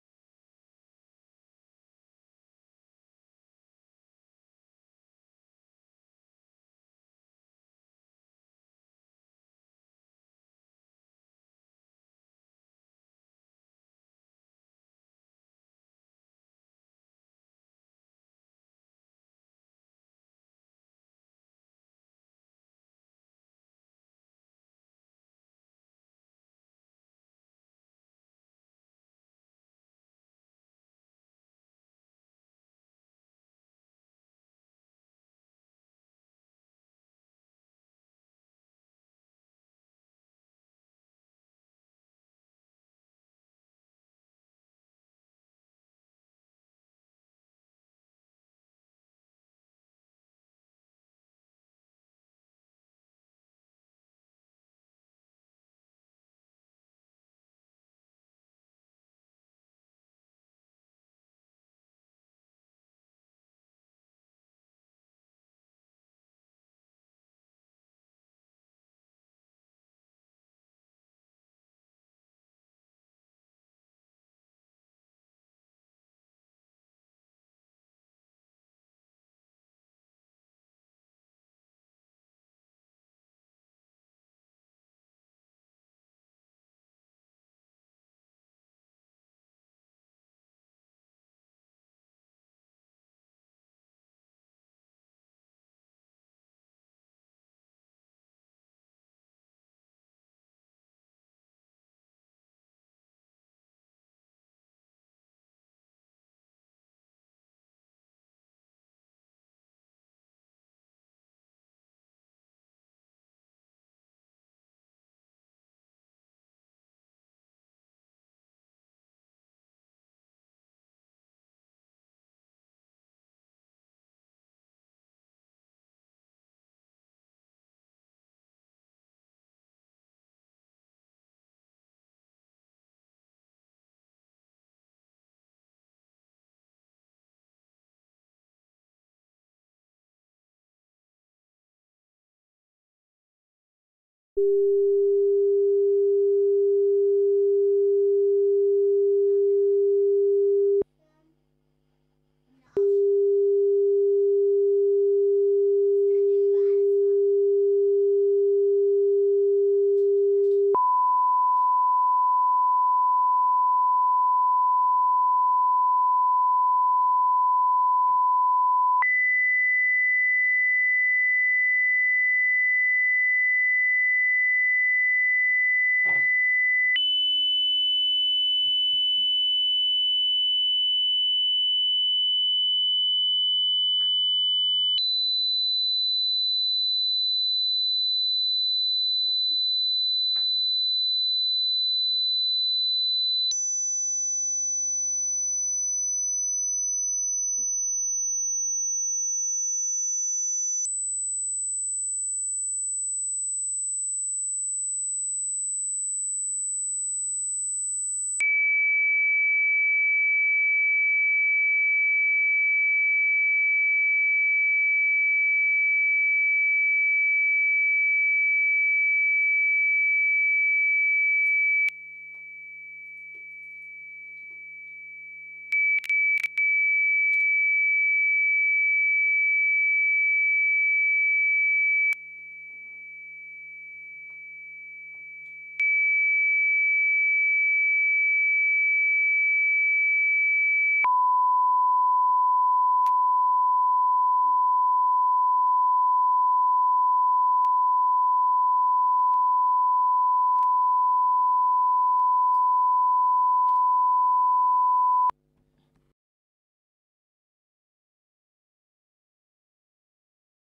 Bandsperre im Audiokabel
In ein handelsübliches Audiokabel mit 3,5 mm Klinkenstecker und -kupplung wird zunächst eine Spule (L=47,3 Milli-Henry) eingebaut. Das Verhalten bei verschiedenen Frequenzen wird untersucht.
Zusammen mit einem parallel zur Spule geschalteten Kondensator (C=0,1 Mikro-Farad) entsteht ein Schwingkreis, der im Bereich der Resonanzfrequenz eine Sperre darstellt.